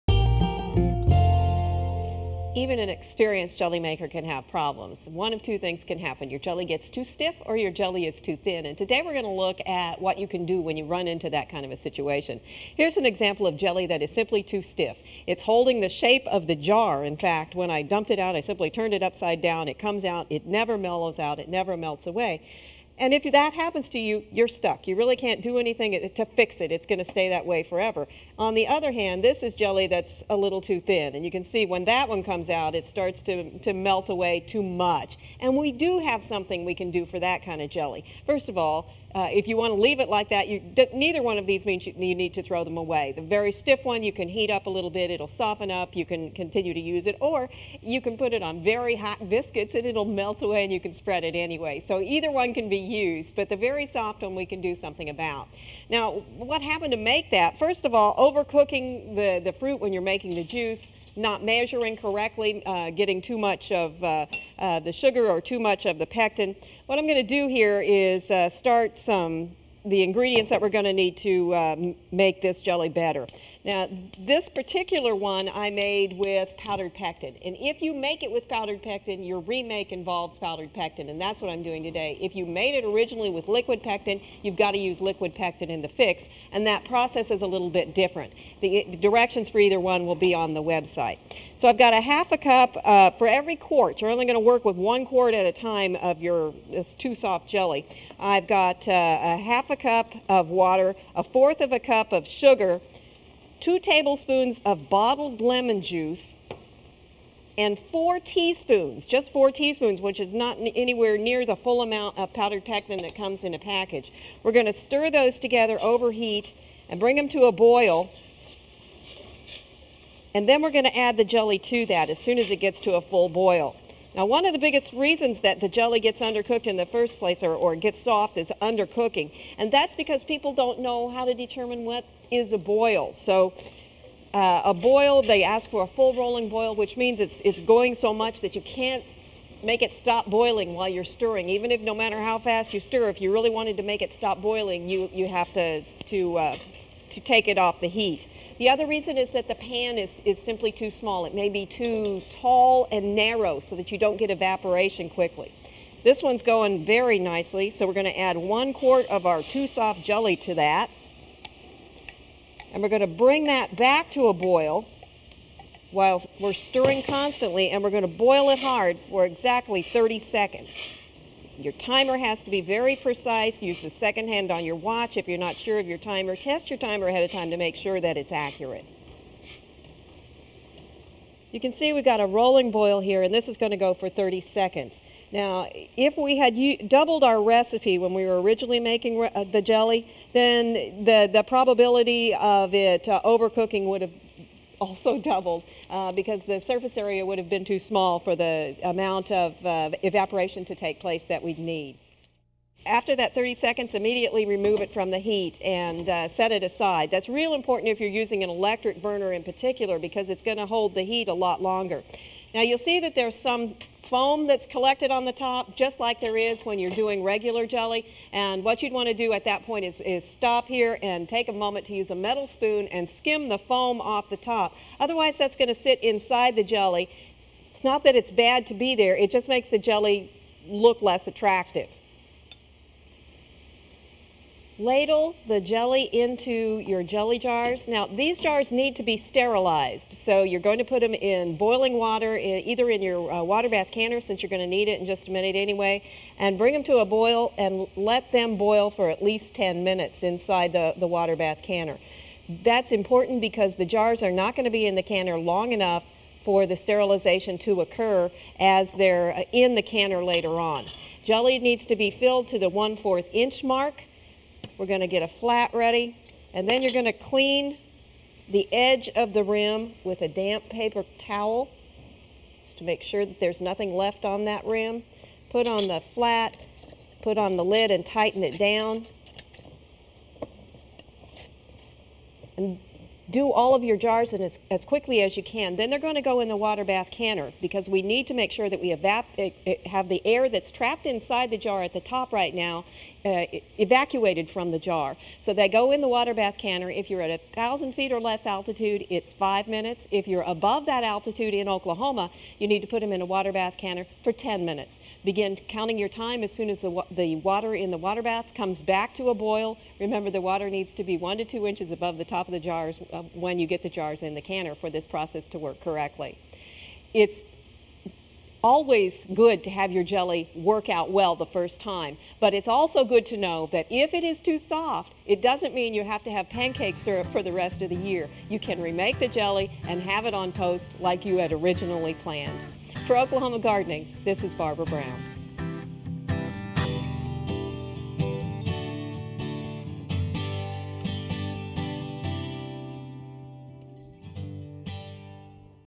Jelly
(9/17/05)-Extension Food specialist Barbara Brown demonstrates how to make jelly perfect, not too thin and not too stiff, every time.